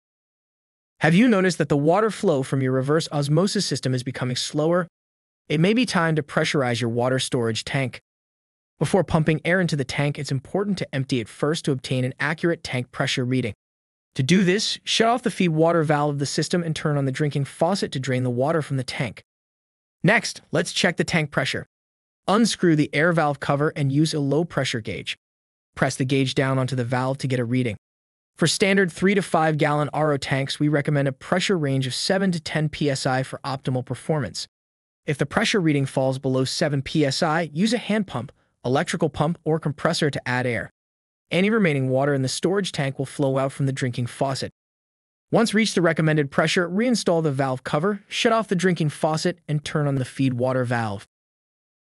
Boost Your RO System's Water Flow: Fix Slow Flow with Tank Pressure!
Having trouble with your RO system's water flow? Don't worry, we've got you covered! In this concise and actionable YouTube Shorts video, we address the issue of slow water flow and share a quick fix. Watch as we guide you through the process of measuring the pressure in your RO system's tank. Discover the magic pressure range that ensures optimal water flow and learn how to pump up the pressure for remarkable results. Get ready to experience the difference as your RO system delivers a steady and satisfying flow of purified water!